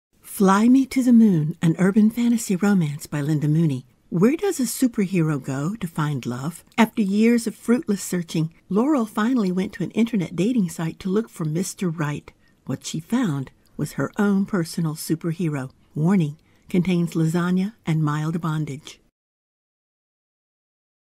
FLY ME TO THE MOON, a Sensuous Urban Fantasy Romance
Urban Fantasy Romance
Written and Narrated by Linda Mooney
Word Count: 13.4K
Length: 1 hr, 38 min
$1.99 e / $7.50 p / $6.04 a